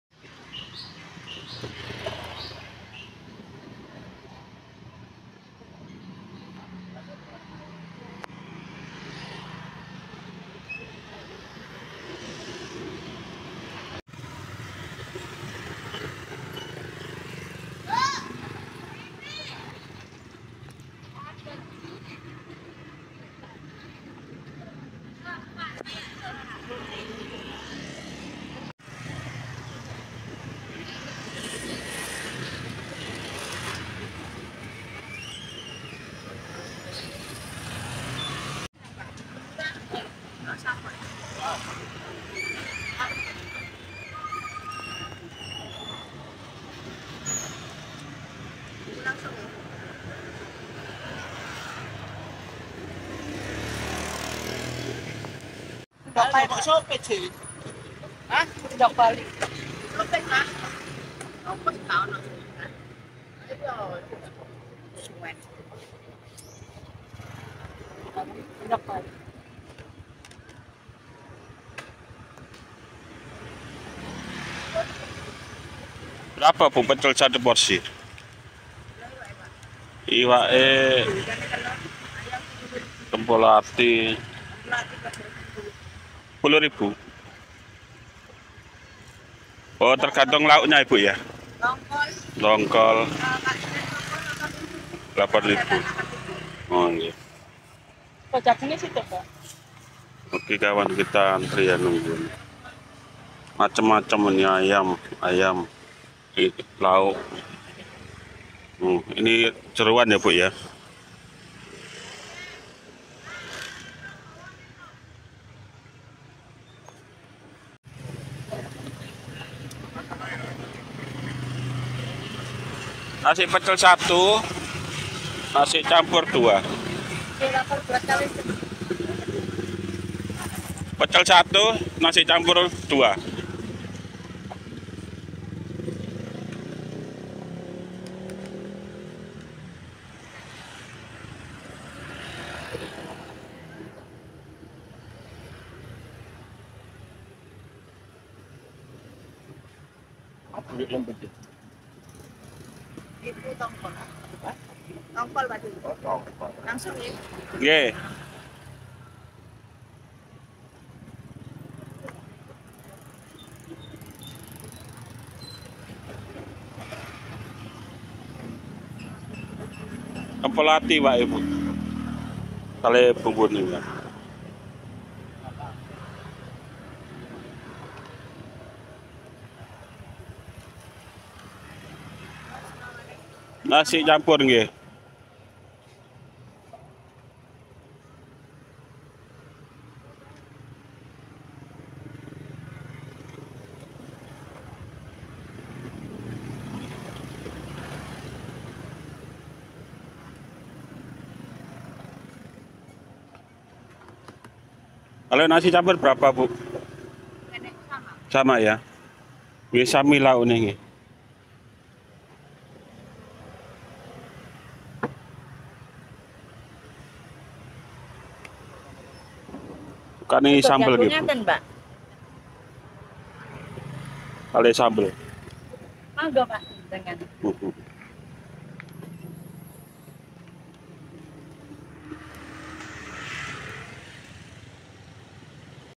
Nasi Campur dan Pecel Bu Rima Surabaya | Daily Vlog
Kuliner Jalanan yang ada di Jojoran gang 1 Surabaya, Bu Rima Menjual Nasi Pecel, Nasi Campur dan Nasi Jagung yang murah meriah dan enak rasa nya, Yuk Cekidot